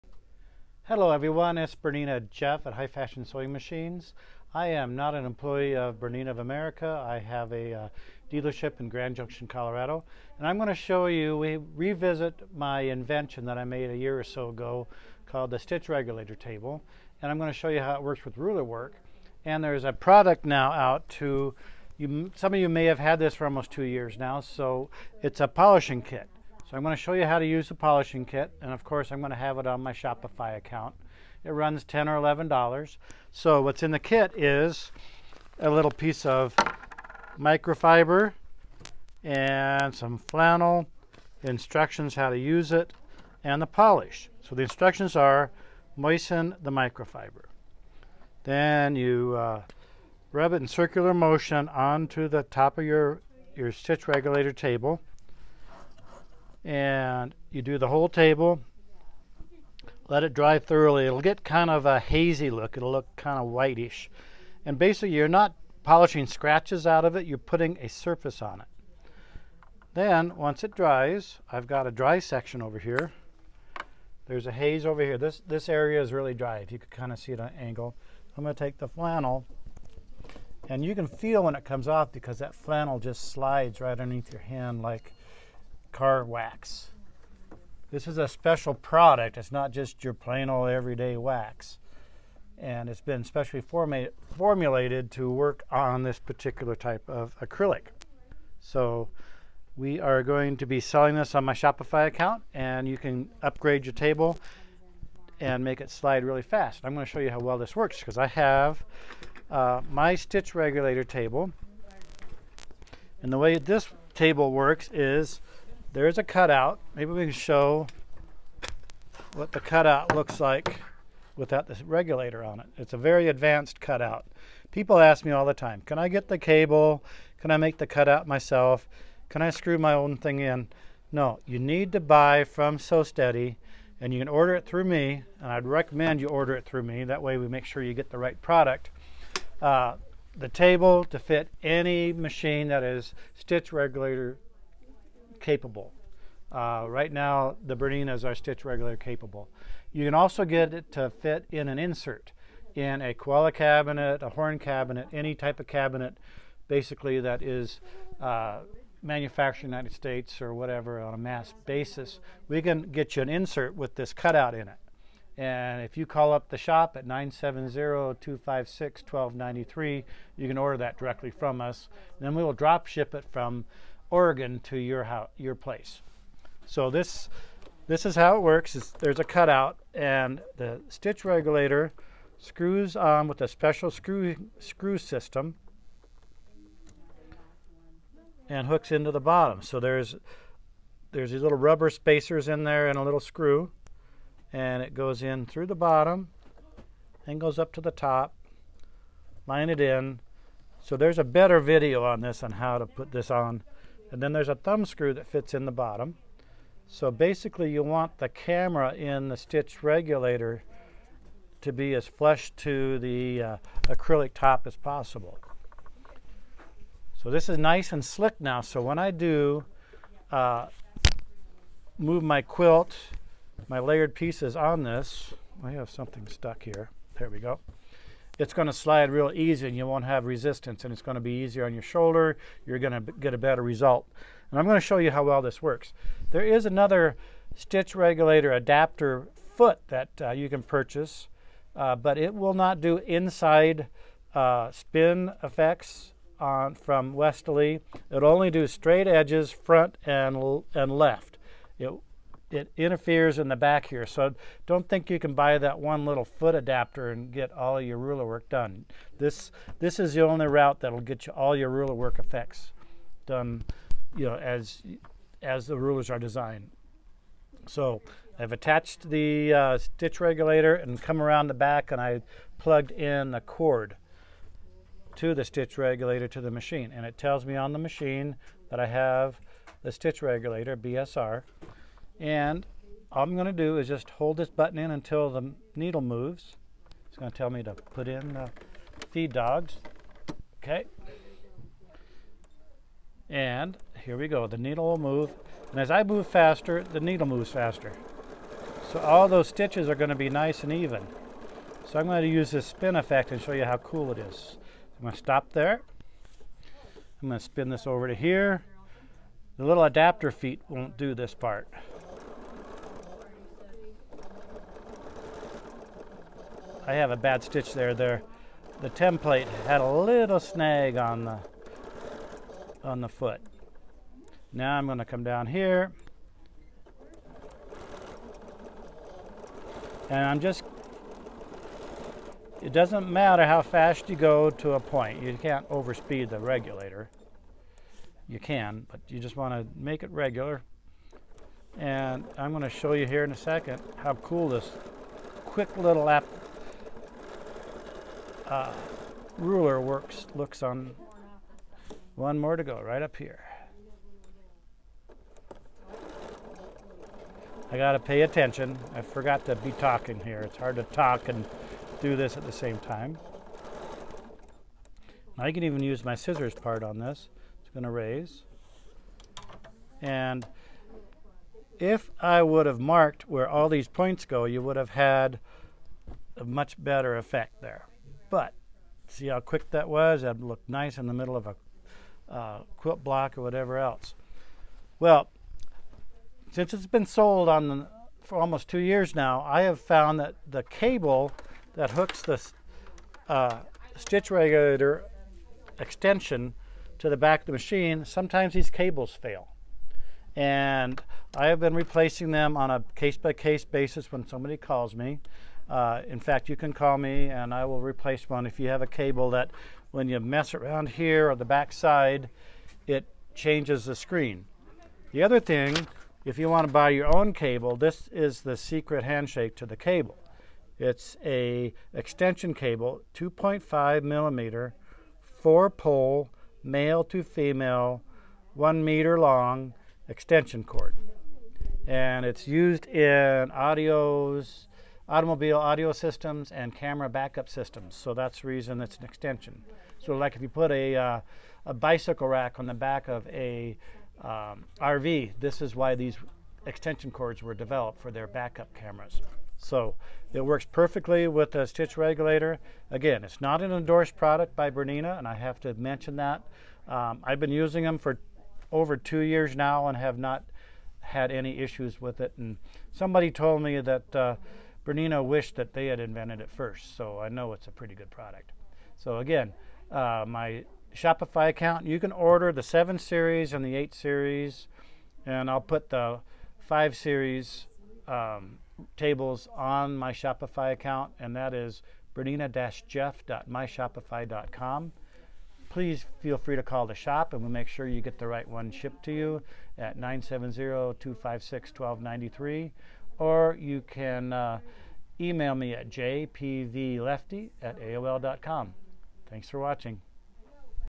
Ruler Work Stitch Regulator table invention new polish to make it slick how to get a new cable Jeff
Bernina Jeff Sew Tech Jeff shows how to use his invention Ruler Work Stitch Regulator table invention new polish to make it slick how to get a new cable Jeff
Bernina-jeff.myshopify.com